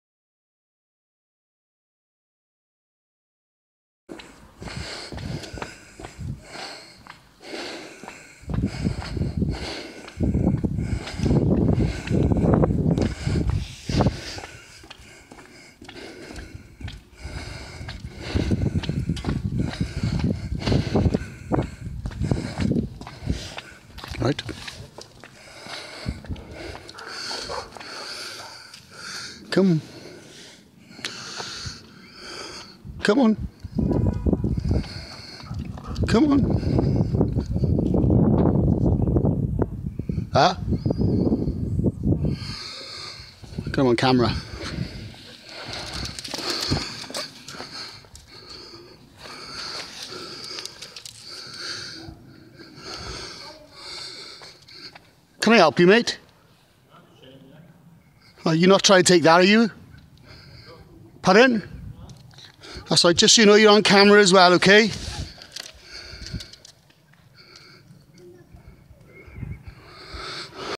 Suspicious Person Caught With EE Action Cam
Alleged Suspect Caught on EE Action Cam today. whilst out walking the dog, I was trying a new go pro head strap with my EE Action Camera. This chap in the video was seen by my neighbours to be ALLEGEDLY trying to take items from a neighbours driveway AND peering over a 6ft wall into their garden. aint Tech Awesome?? this has been reported to the Police with the images and video.. ill update if I get any more info